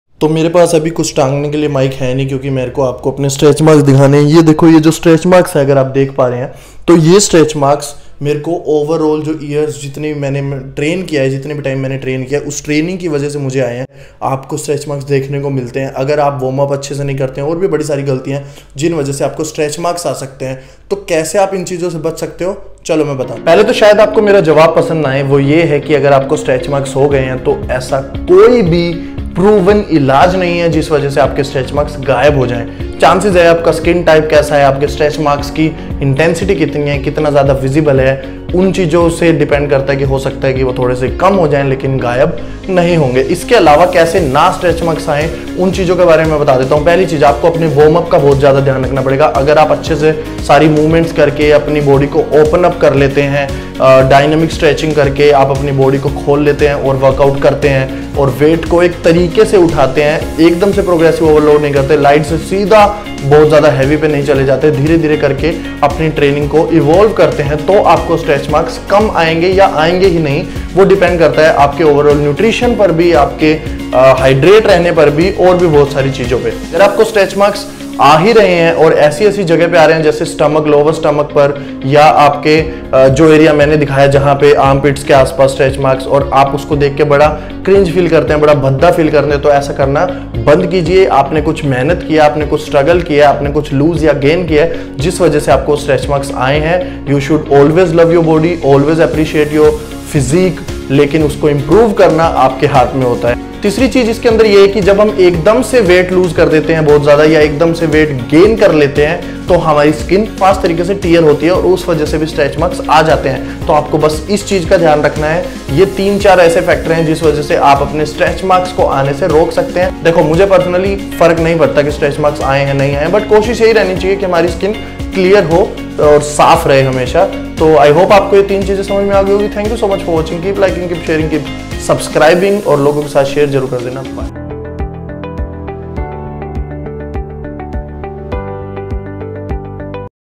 How to decrease STRETCH MARKS? स्ट्रेच मार्क्स कैसे कम करें? || Deepak Kaushish
Stretch marks can have causes that aren't due to underlying disease. Examples include pregnancy, weight gain in obesity, growth spurts in teenagers, medication side effects or a rapid increase in muscle size through weightlifting.

खिंचाव के निशान के ऐसे कारण हो सकते हैं जो अंतर्निहित बीमारी की वजह से नहीं हों. उदाहरणों में शामिल हैं गर्भावस्था, मोटापे में वजन बढ़ना, किशोरों में वृद्धि में तेजी, दवा के दुष्प्रभाव या भारोत्तोलन के माध्यम से मांसपेशियों के आकार में तेजी से वृद्धि।